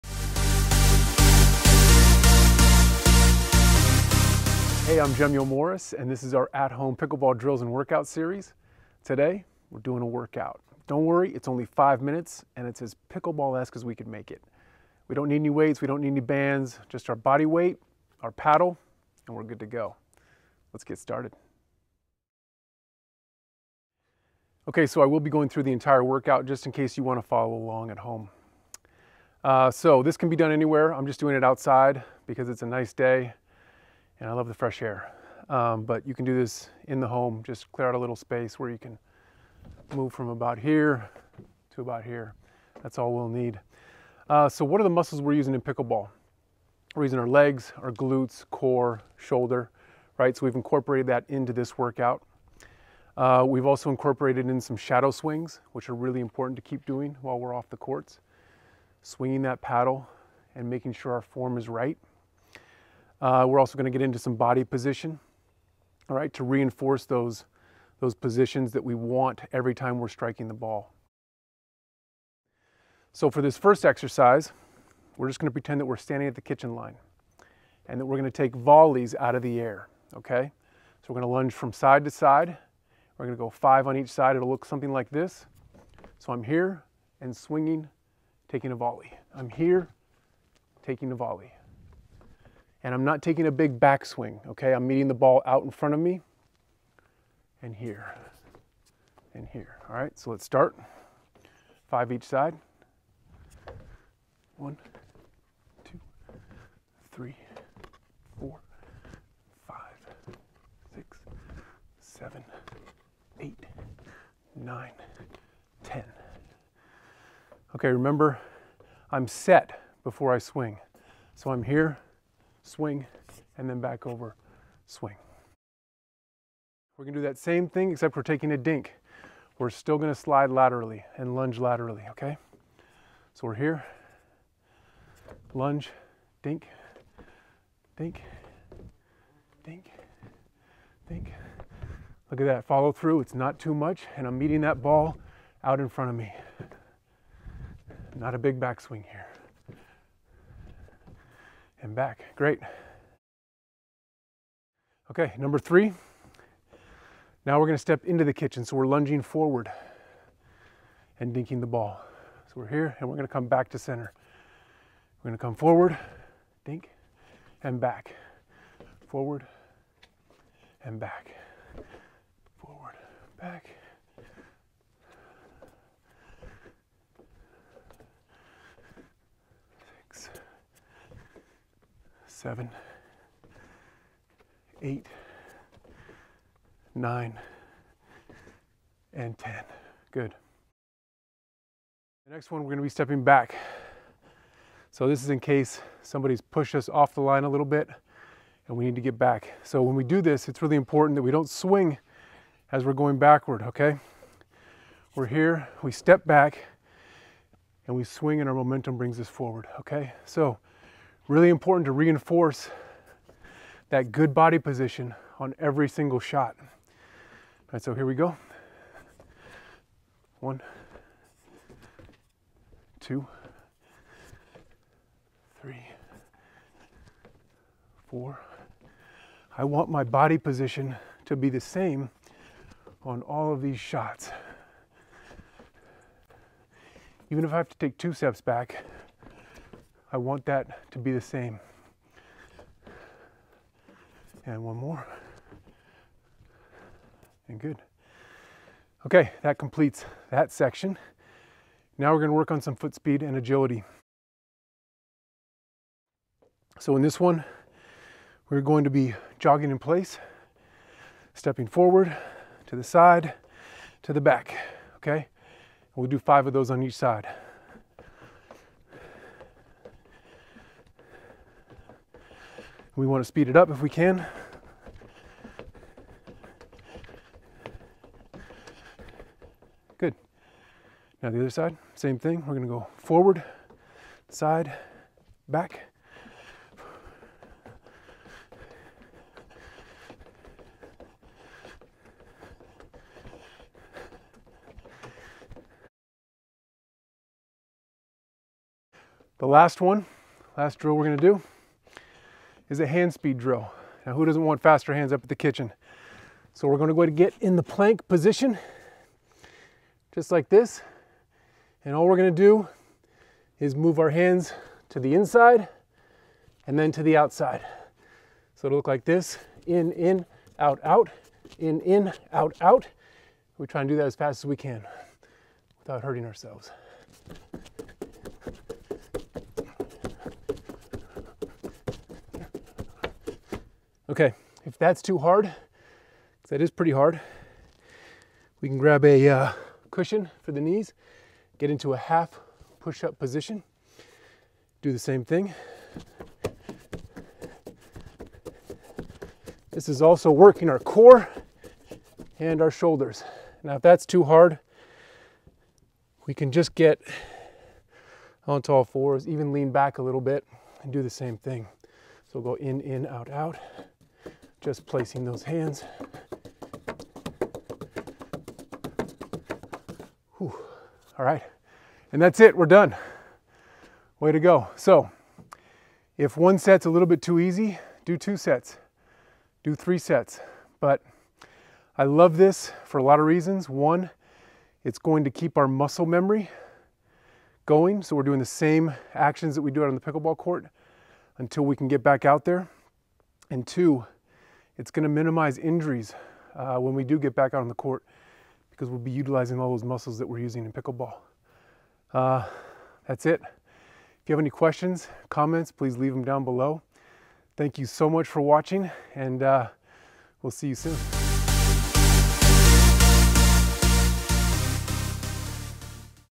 5 Minute Pickleball Workout
Session #2 of our, “At home pickleball drills and workouts” Series. For most of us, pickleball IS our workout. So this workout is for when we can’t make it to the court and we still want to keep in pickleball shape.
This 5 minute workout can be done in the home, outside, at your hotel or wherever you happen to be without pickleball.